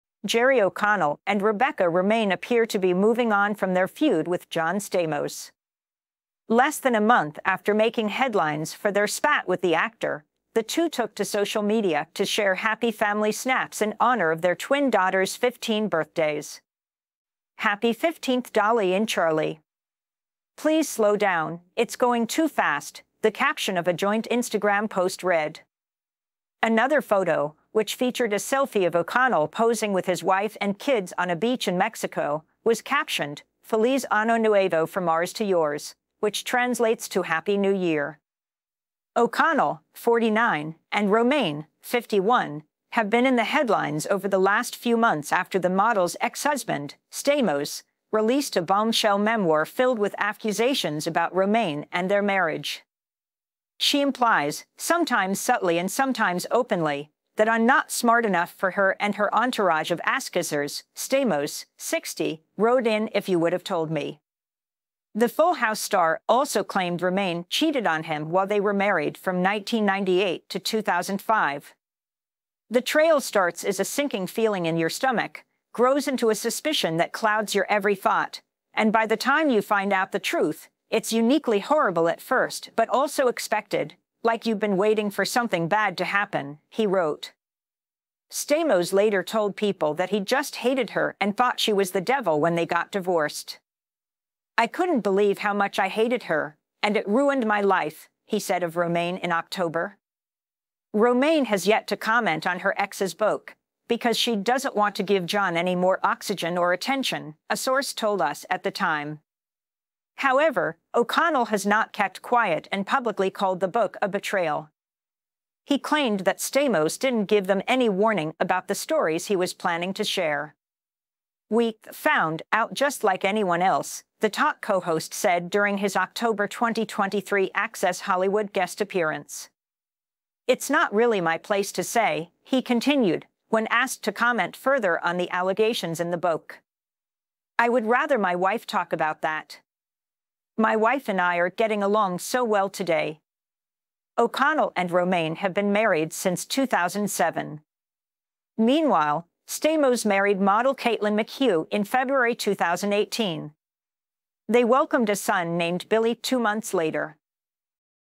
Jerry O’Connell Rebecca Romijn pose for beach photo with twins after John Stamos feud
The married duo have been feuding with Romijn’s ex-husband ever since the “Full House” star released a memoir and made accusations against the model.